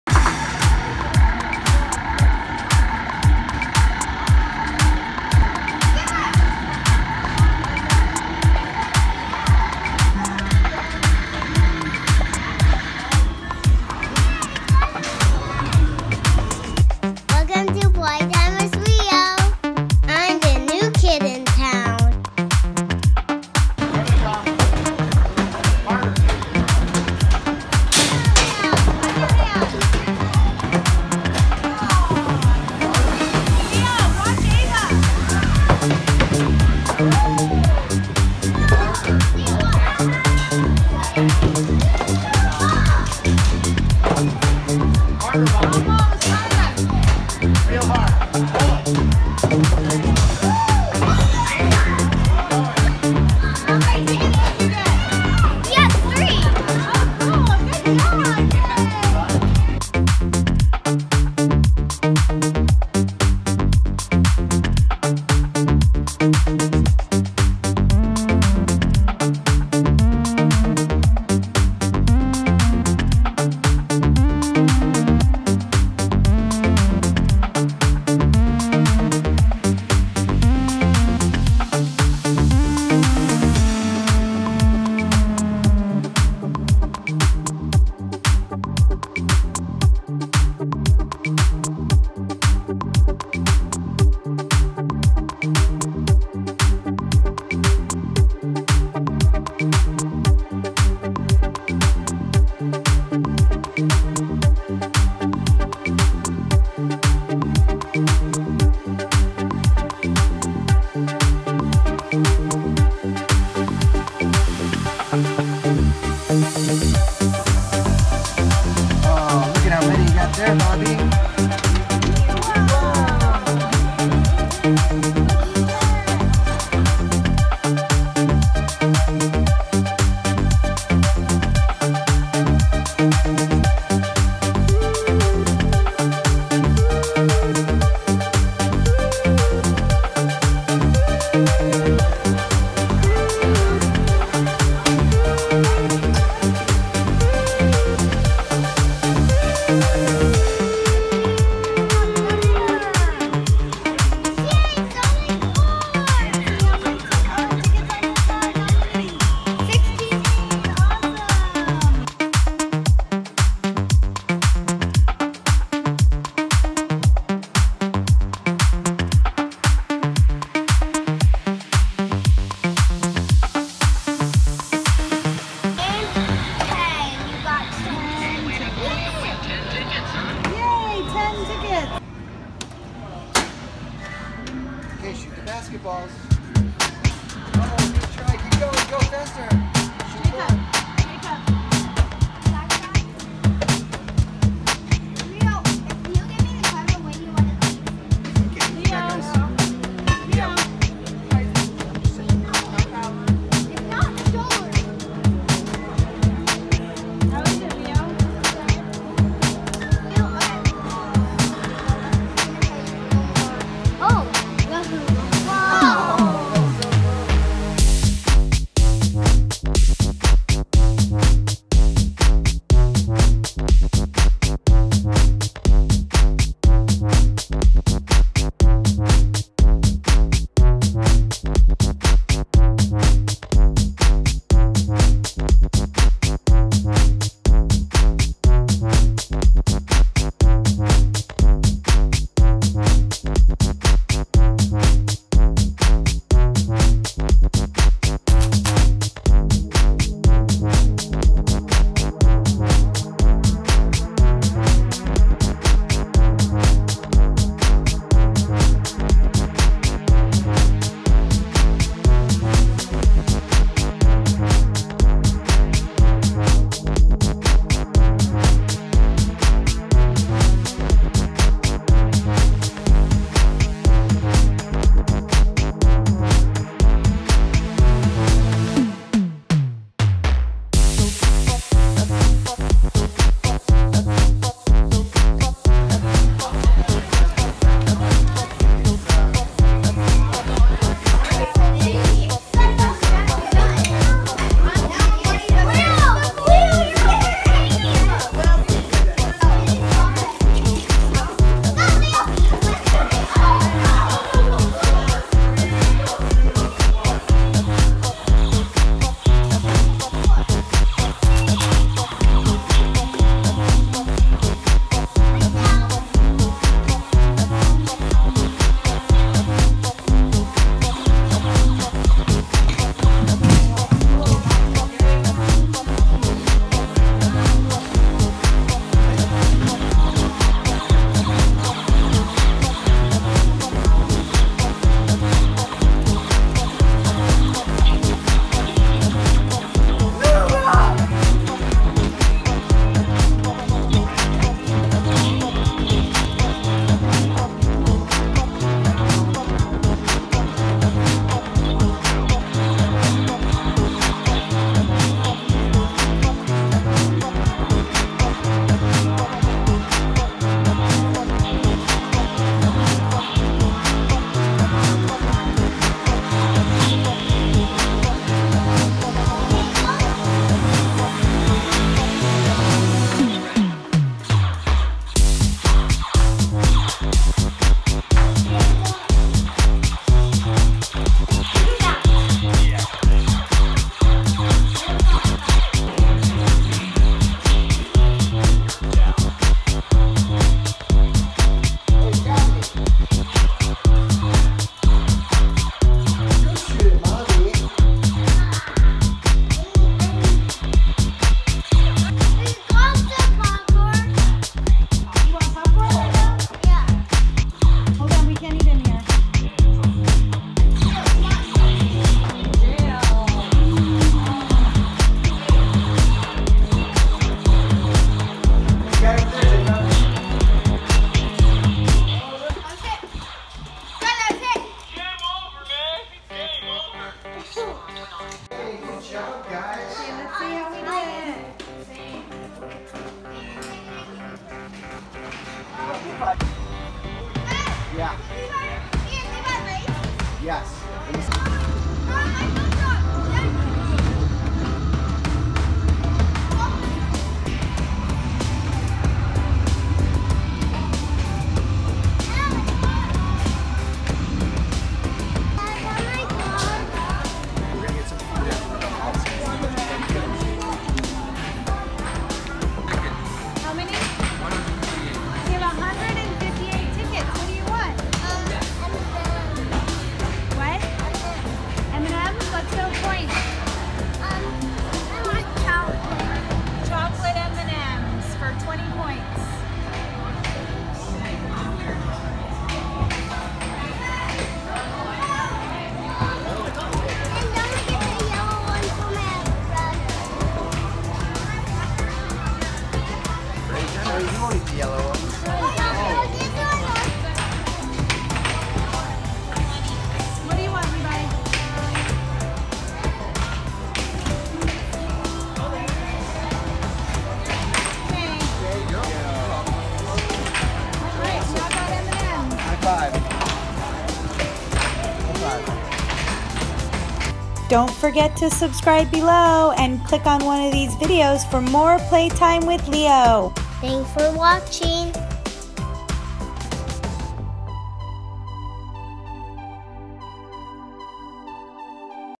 Laser Tag, Indoor Ropes Course and Arcade Games!
Watch Leo and his brothers explore Laser Oasis. Arcade games, Indoor Ropes course and Laser Tag fun. So much fun filled in one place!